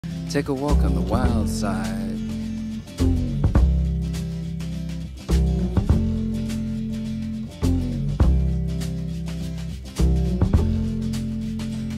Painting wildlife using Procreate on my iPad
Painting the elusive Bittern who is perfectly camouflaged using Procreate… 🪶